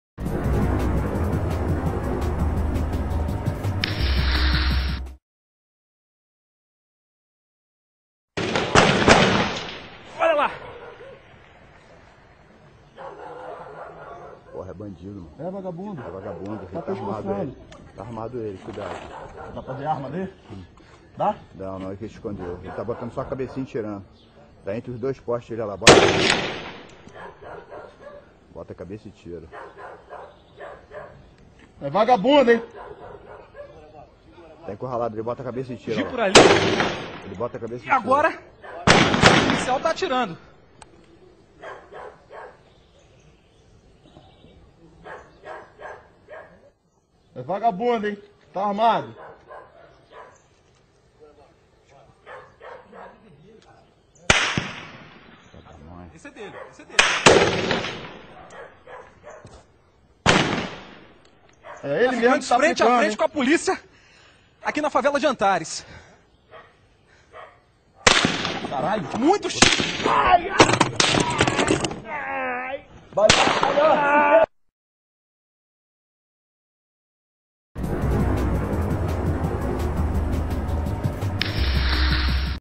Cinegrafista morre em Antares
Cinco pessoas morreram, entre elas o cinegrafista da TV Bandeirantes Gelson Domingos da Silva, 46 anos,  em operação da PM na Favela de Antares, em Santa Cruz, Zona Oeste. As outras quatro, segundo a polícia, eram traficantes. Atingido por um tiro de fuzil no tórax, Gelson foi levado à UPA do Cesarão, em Santa Cruz, mas já chegou morto. A vítima usava o único modelo de colete autorizado pelas Forças Armadas e que foi atravessado pelo projétil.